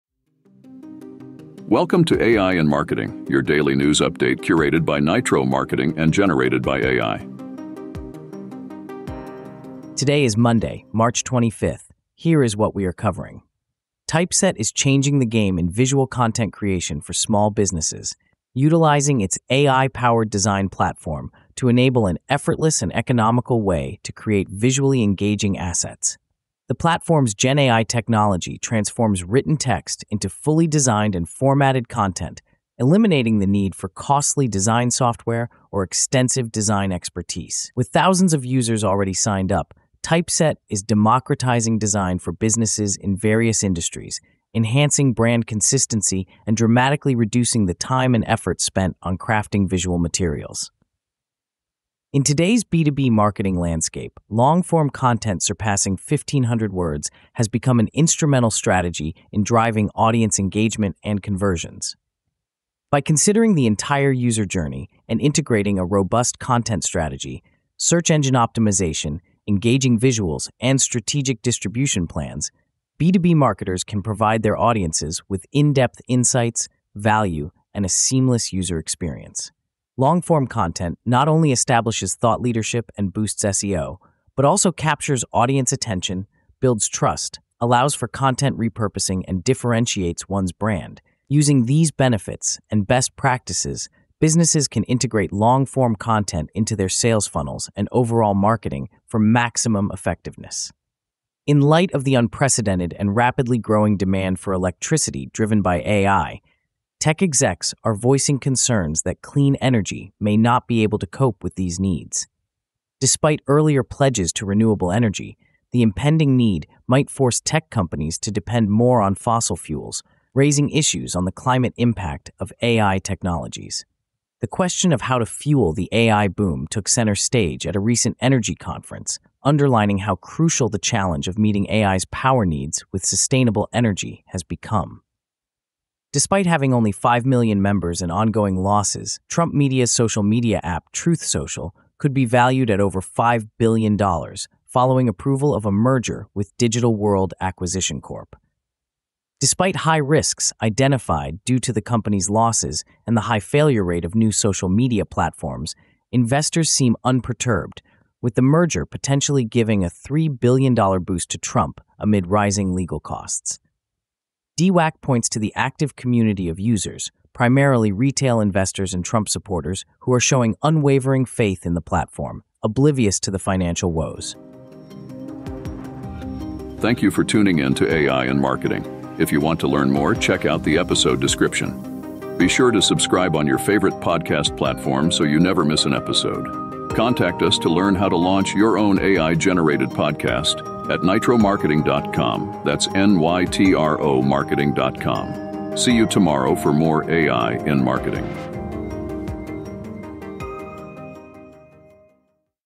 "Typeset Transforms Content Creation, The Power of Long-form B2B Content, AI's Energy Challenge, ...
On "AI in Marketing with Nytro Marketing," today, we discuss how Typeset's AI-powered design platform is revolutionising visual content creation for small businesses. We also share insights on why long-form content of over 1,500 words is gaining traction in the B2B marketing landscape. Additionally, we shed light on the rising concerns amongst tech executives about renewable energy's capacity to cope with the massive electricity demand driven by AI technology, which might force the industry to depend more on fossil fuels. Lastly, we discuss the high valuation of Trump Media's social media app, Truth Social, despite the company's ongoing losses and limited user base.